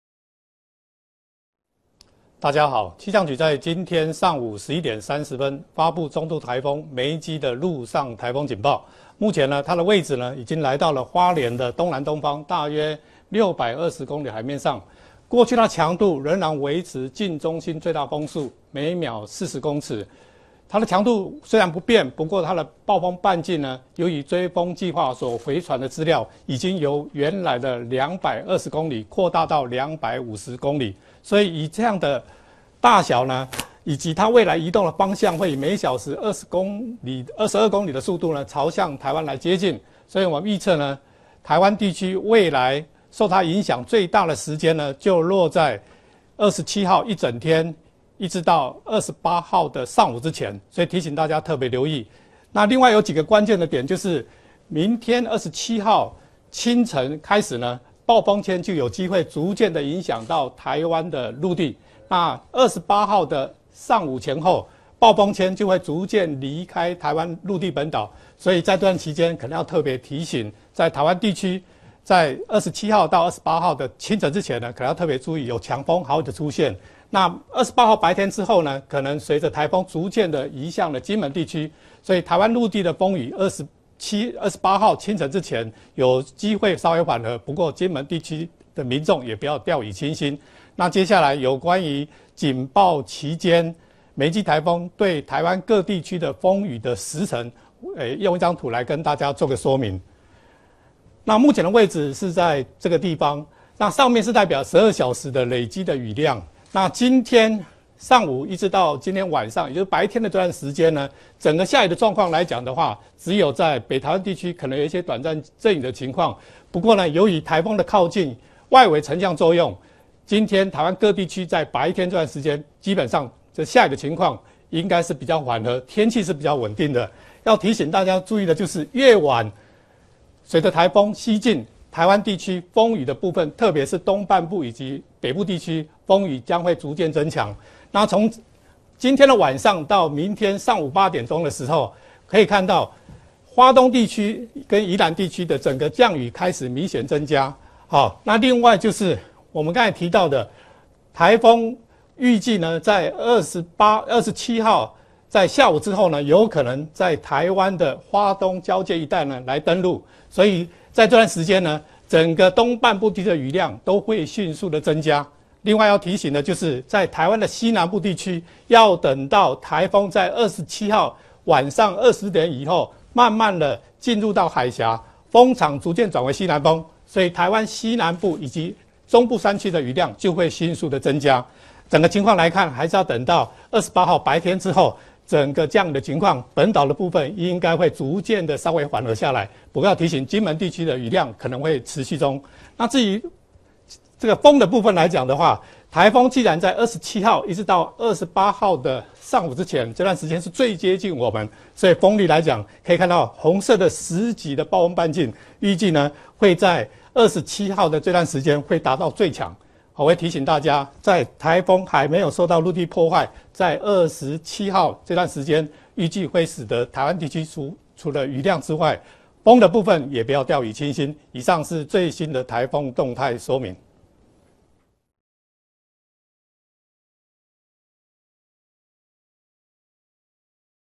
梅姬暴風圈略擴大 氣象局11：30發陸警
中度颱風梅姬逐漸靠近台灣，中央氣象局在今天（26日）上午11時30分發布陸上颱風警報，指颱風暴風圈略為擴大，7級風暴風半徑從220公里增至250公里，而花蓮、宜蘭及台東（含蘭嶼、綠島）地區應嚴加戒備，並防強風豪雨。